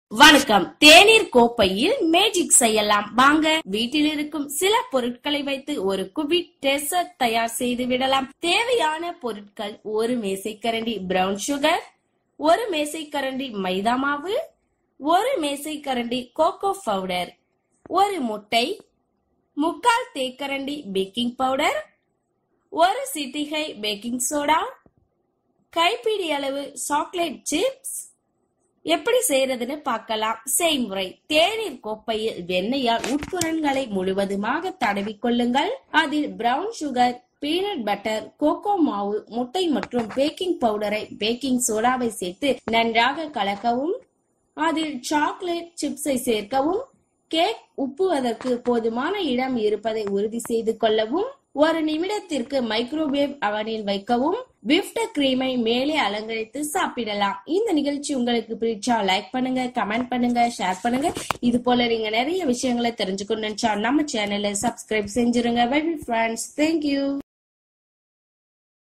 தேனீர் கோப்பையில் மேஜிக்
A Creative video by Samayal Samayal

twitter இருக்கும் சில பொருட்களை வைத்து ஒரு குவிக் டெசர்ட் (இனிப்பை) தயார் செய்துவிடலாம்.

தேவையான பொருட்கள்:
- 1 மேசைக்கரண்டி பிரவுன் சுகர்
- 1 மேசைக்கரண்டி மைதா மாவு
- 1 மேசைக்கரண்டி கோக்கோ பவுடர்
- 1 முட்டை
- 1/4 தேக்கரண்டி பேக்கிங் பவுடர்
- ஒரு சிட்டிகை பேக்கிங் சோடா (தேவைப்பட்டால்)
- கைப்பிடியளவு சாக்லேட் சிப்ஸ்

செய்முறை:
தேனீர் கோப்பையை, வெண்ணெயால் உட்புரன்களை முழுவதுமாக தடவிக்கொள்ளவும்.
அதில், பிரவுன் சுகர், பீனட் பட்டர், கோக்கோ, மாவு, முட்டை மற்றும் பேக்கிங் பவுடரை, பேக்கிங் சோடாவை சேர்த்து, நன்றாக கலக்கவும். அதில் சாக்லேட் சிப்ஸைக் சேர்க்கவும். கேக் உப்புவதற்கு போதுமான இடம் இருப்பதை உறுதிசெய்து கொள்ளவும்.
ஒரு நிமிடத்திற்கு மைக்ரோவேவ் அவனில் வைக்கவும். விப்ட் கிரீமை மேலே அலங்கரித்து சாப்பிடலாம்.